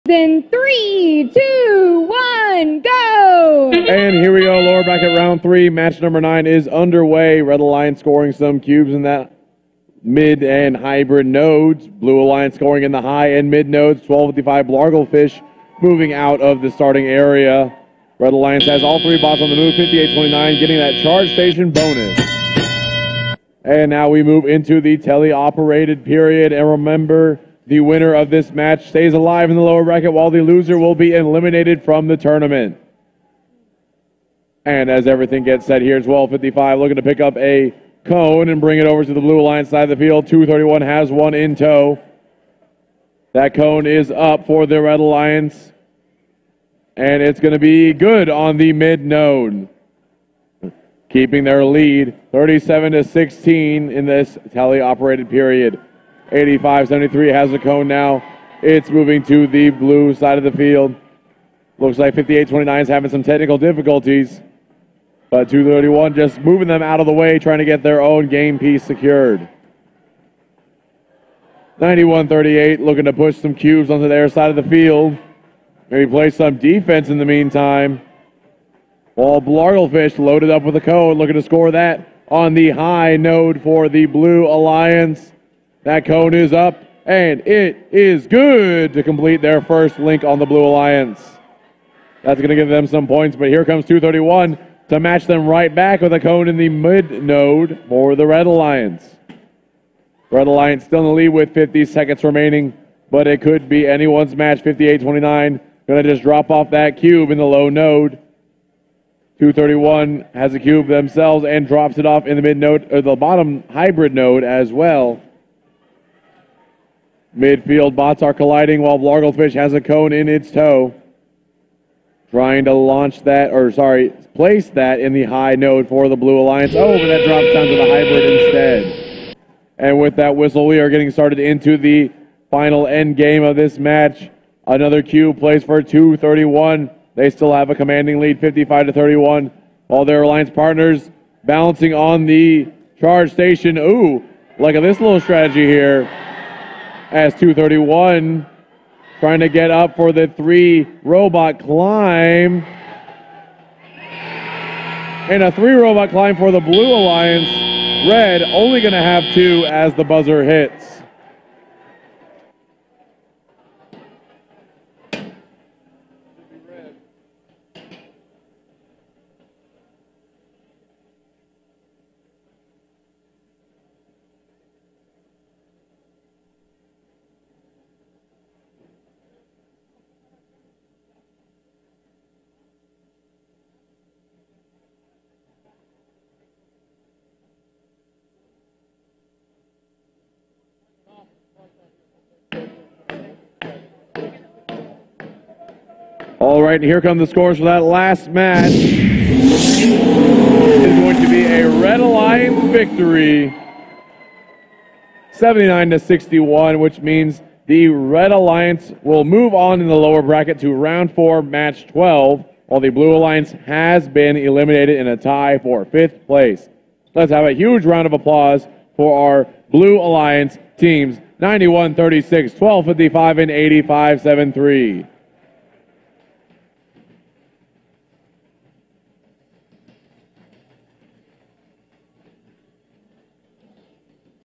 Channelview 2023 M9
Elimination Match 9 - Channelview District Event 2023
 FIRST Robotics Competition
Date: 3/12/2023